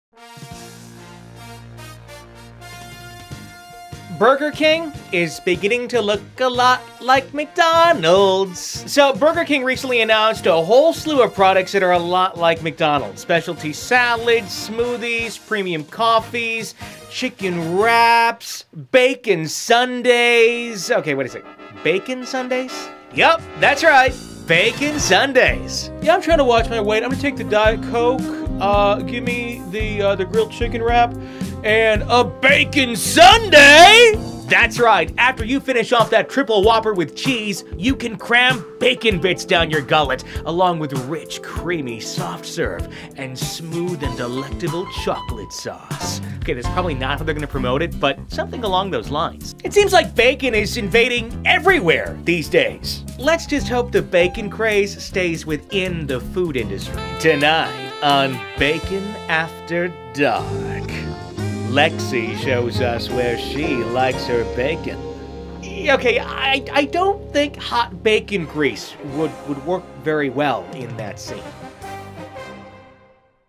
The Bacon Sundae from Burger King
It's here the Bacon Sundae from Burger King. Nothing like soft serve ice cream, chocolate and bacon.